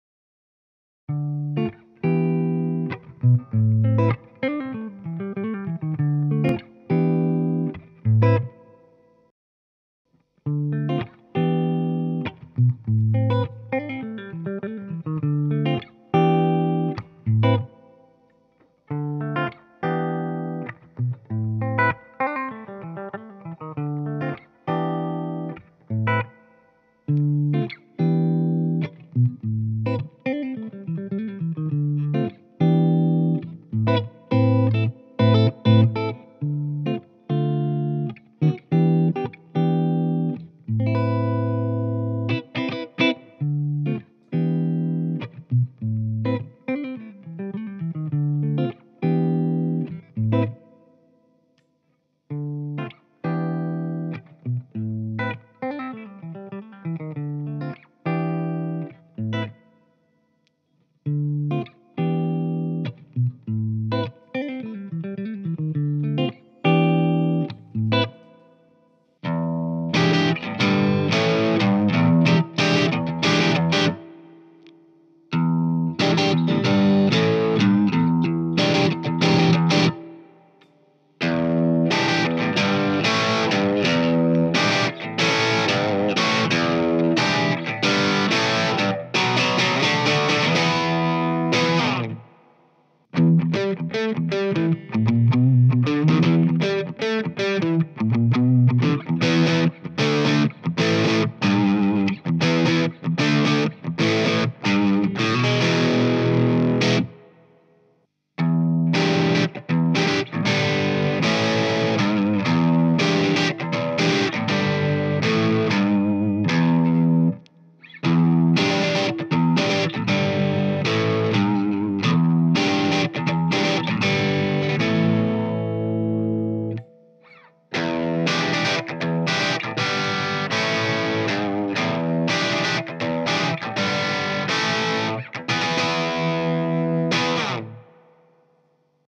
ARB Bluesbacker pickups demo (with removing screws)
This is a review of ARB Bluesbucker pickups. In some samples I removed screws from them.

Gear:
Gibson Les Paul Custom Replica with ARB Bluesbackers
Okko Diablo
Hiwatt Studio/Stage MKIII + TRM VS112TO Cab
Shure SM57